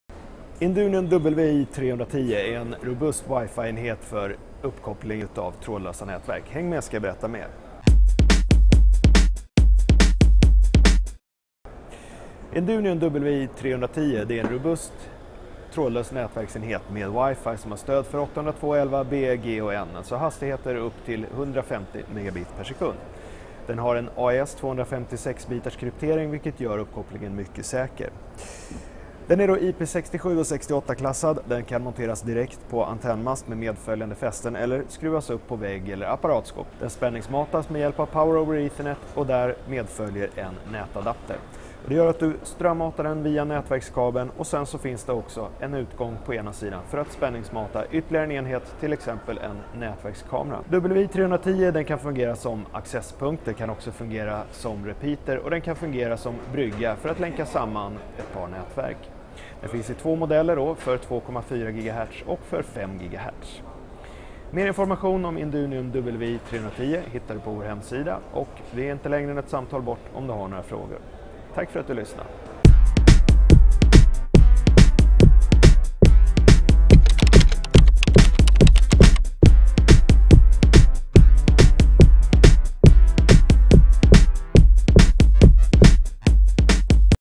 Cerio OW-310N2 industriell acesspunkt och repeater
Industriell wifilösning som fungerar som accesspunkt, repeater eller brygga. Vatten och dammtät och avsedd för utomhusmontage. Hög säkerhet och robustkonstruktion. Med Power over Ethernet in- pch ut.